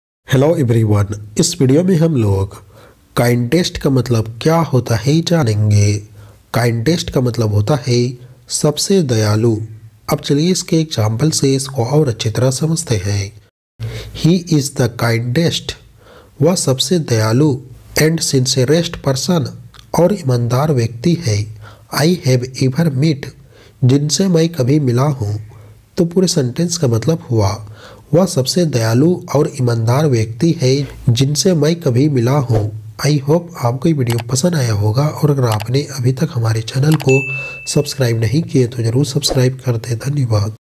Kindest Meaning in Hindi | Kindest ka Matlab kya hota hai | Word Meaning English to Hindi
Kindest Meaning in Hindi | Meaning of Kindest | Kindest का हिंदी में मतलब | English to Hindi vocabulary |

Aaj ki Is Video Me Maine Aapko Kindest Ka Matlab Kya hota hai Hindi mein, Kindest Ka definition or Kindest Word Ke Sentence ka Example ko batlaya hai.